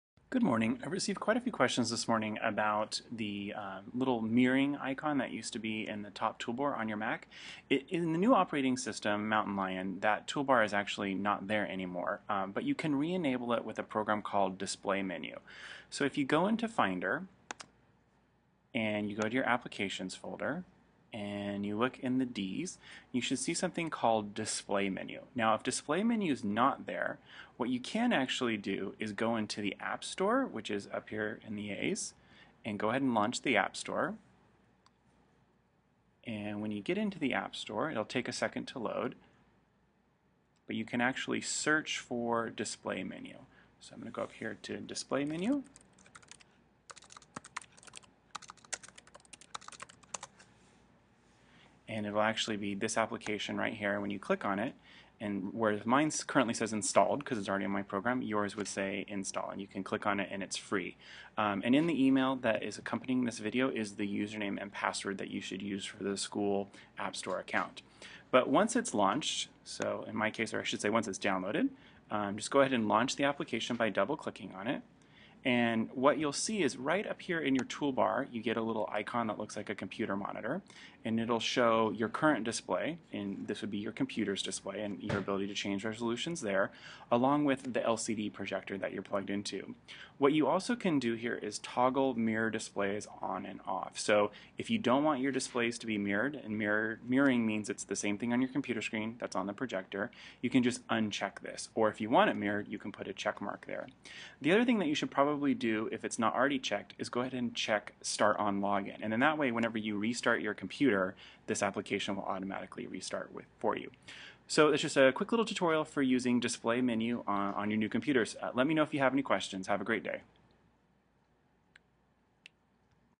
Using the Display Menu App
Using the Display Menu app to restore your Mirroring Icon.